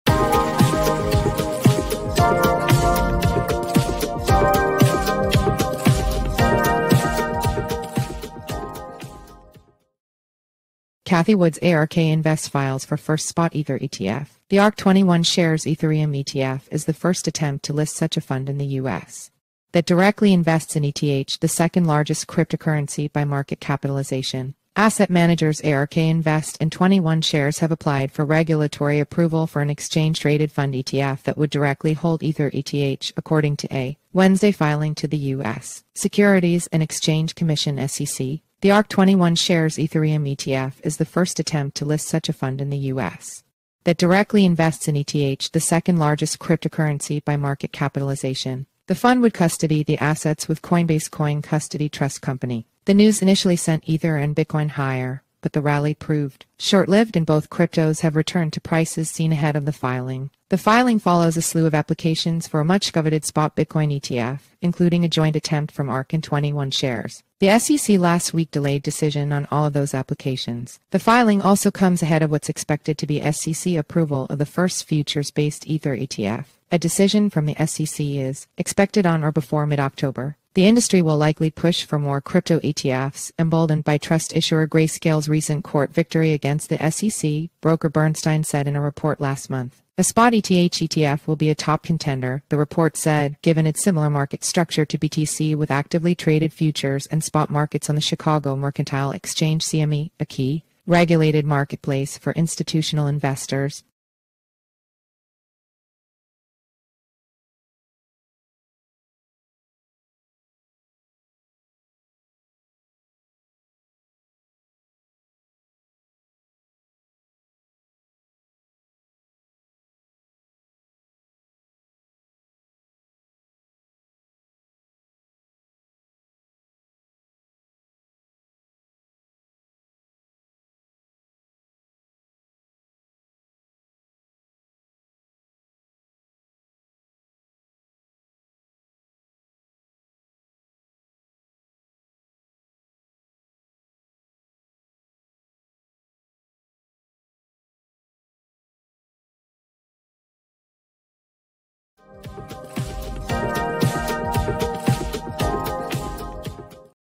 Cathie Wood's ARK Invest Files for First Spot Ether ETF
The Ark 21Shares Ethereum ETF is the first attempt to list such a fund in the U.S. that directly invests in ETH, the second largest cryptocurrency by market capitalization.

Asset managers ARK Invest and 21Shares have applied for regulatory approval for an exchange-traded fund (ETF) that would directly hold ether (ETH), according to a Wednesday filing to the U.S. Securities and Exchange Commission (SEC).

The fund would custody the assets with Coinbase (COIN) Custody Trust Company.

The news initially sent ether and bitcoin higher, but the rally proved short-lived and both cryptos have returned to prices seen ahead of the filing.

The filing follows a slew of applications for a much-coveted spot bitcoin ETF, including a joint attempt from Ark and 21Shares. The SEC last week delayed decision on all of those applications.

The filing also comes ahead of what's expected to be SEC approval of the first futures-based ether ETF. A decision from the SEC is expected on or before mid-October.

The industry will likely push for more crypto ETFs, emboldened by trust issuer Grayscale's recent court victory against the SEC, broker Bernstein said in a report last month. A spot ETH ETF will be a top contender, the report said, given its similar market structure to BTC with actively traded futures and spot markets on the Chicago Mercantile Exchange (CME), a key regulated marketplace for institutional investors.